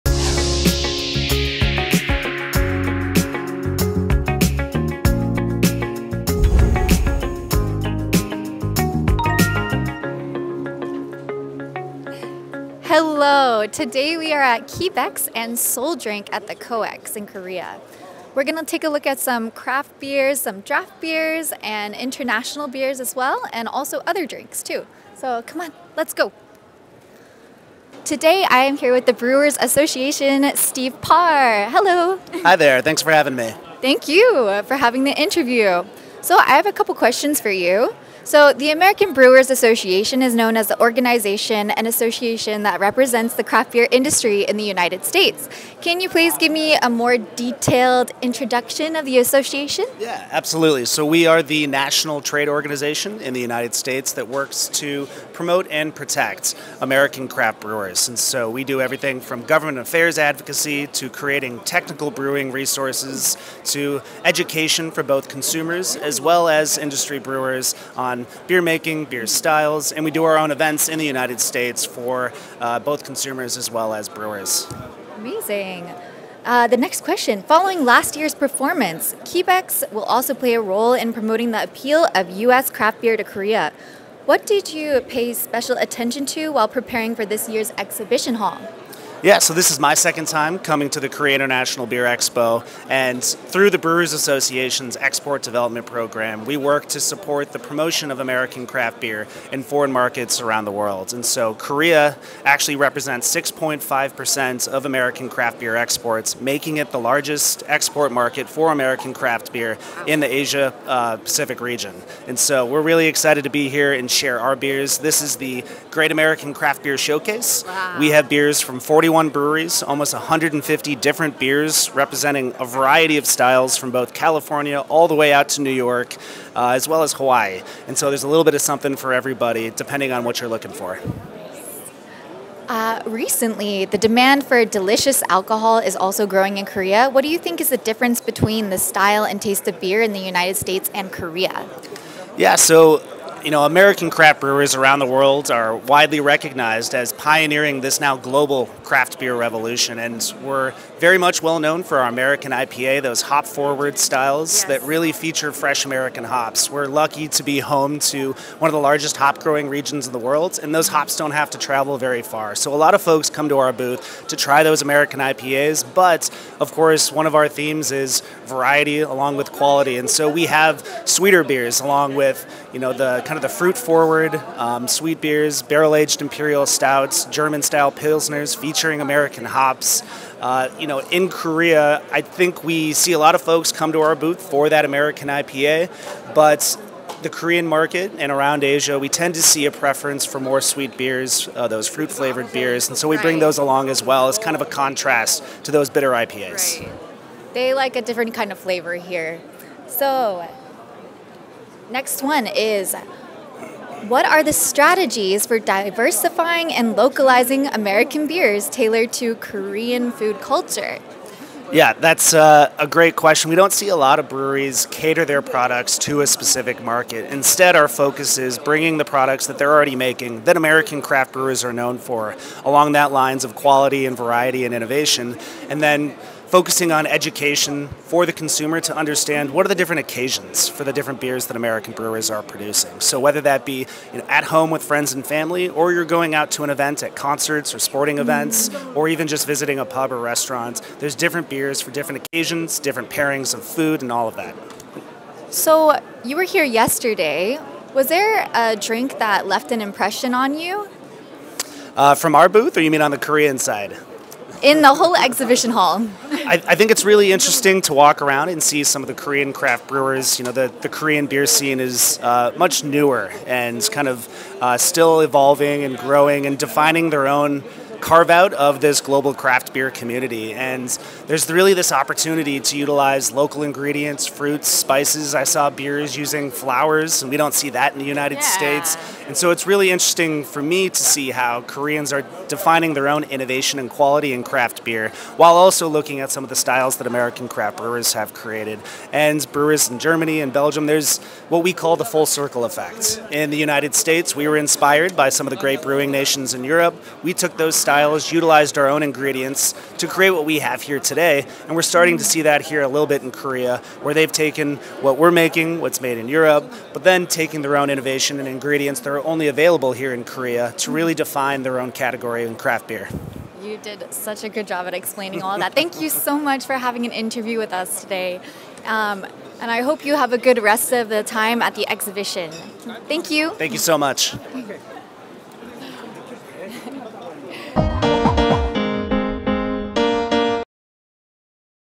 BREWERS ASSOCIATION(BA) 협회 소개
BREWERS ASSOCIATION(BA)은 오는 4월 11일(목)부터 13일(토)까지 3일간 서울 코엑스에서 개최되는 ‘DRINK SEOUL(드링크 서울) 2024’에 참가한다고 밝혔다.

한편, DRINK SEOUL은 프리미엄 전통주, 와인, 증류주 및 무알코올 음료 등 다양한 주류 및 음료산업을 한 자리에서 만날 수 있는 편리한 기회를 제공한다. 전시회 기간 동안 ▲KIBEX(대한민국맥주박람회) ▲KIBA(대한민국국제맥주대회) ▲KIBCON(대한민국국제맥주컨퍼런스)이 동시 개최된다. 아울러 부대행사로는 B2B(1:1 비즈니스 매칭 프로그램), 궁중 술 빚기 대회, 전통주 소믈리에 대회가 마련된다.